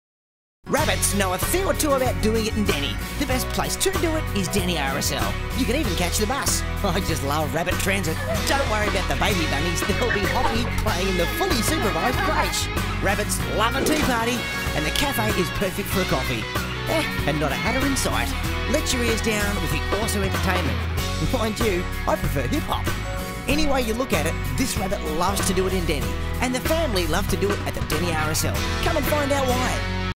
Deni RSL Bunnies
Deniliquin RSL New TVC Campaign

Direction by Nigel Parsons
CGI by Robert Parsons